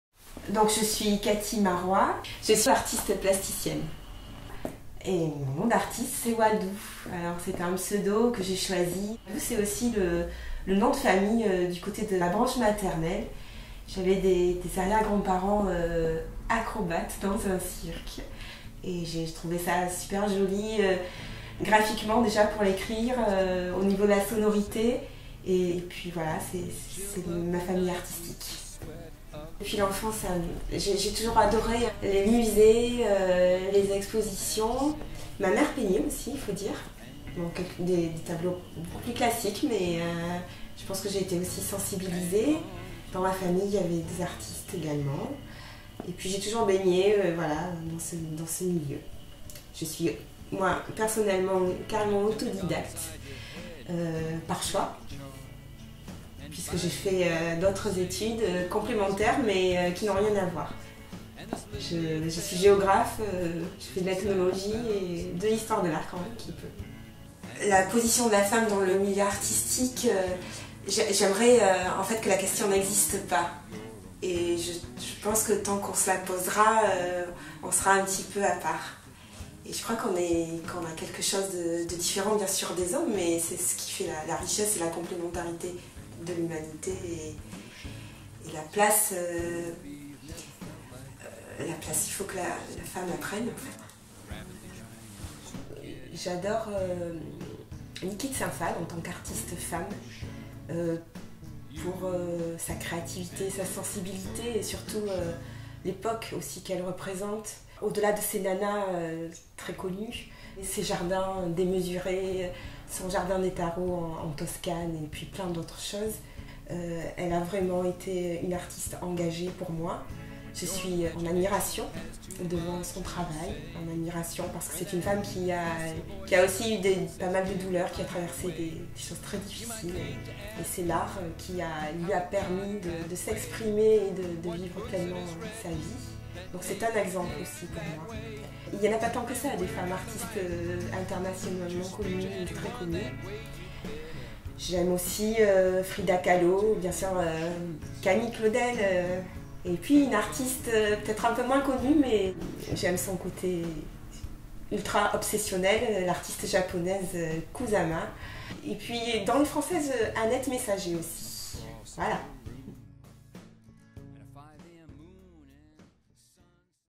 Waldoo (Art Femmes)
Interview de l'artiste Waldoo diffusée lors de l'exposition "Art Femmes", du 19 au 20 mars 2016, dans l'atelier de Jean-François ANDRE à Floirac.